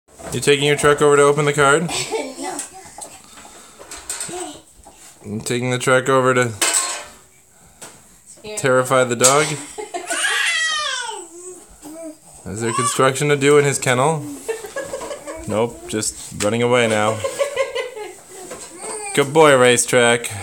Tonka Playtime at the Dog's Expense
Running around with her new truck, Aeryn had determined there is work to do wherever the dog is lying down.